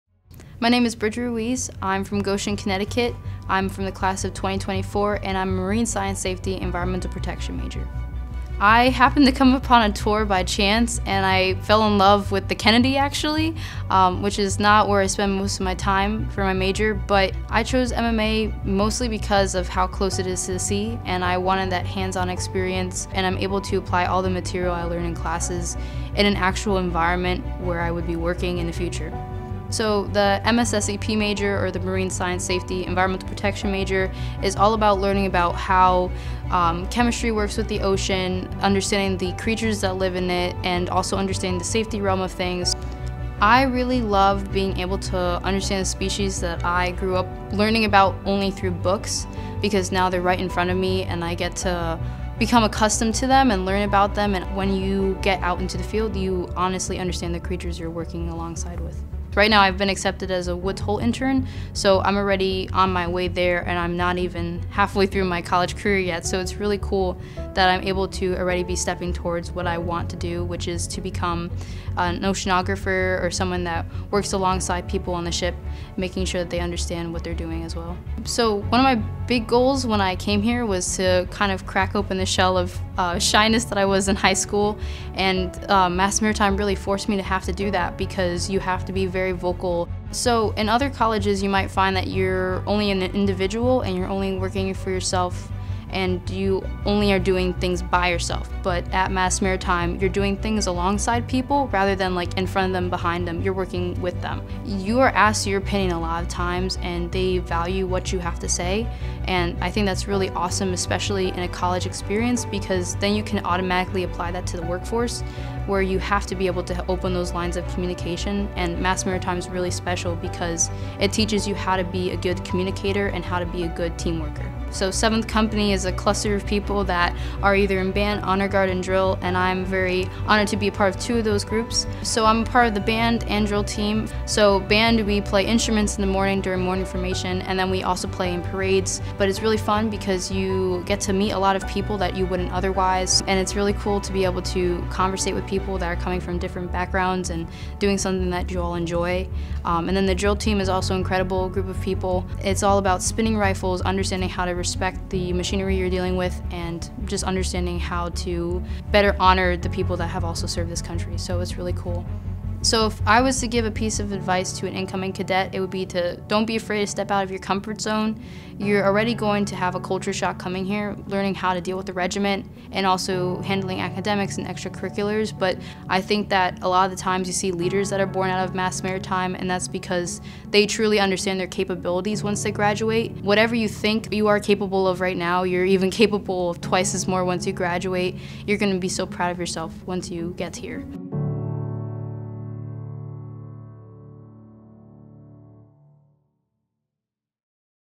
Viewbook Profile: Bridget Ruiz
Major: Marine Science, Safety, Environmental Protection (MSSEP)

“Whatever you think you are capable of right now, you’re capable of twice as much once you graduate. You’re going to be so proud of yourself once you get here.”

Learn more about Massachusetts Maritime Academy and the Marine Science, Safety and Environmental Protection major at maritime.edu.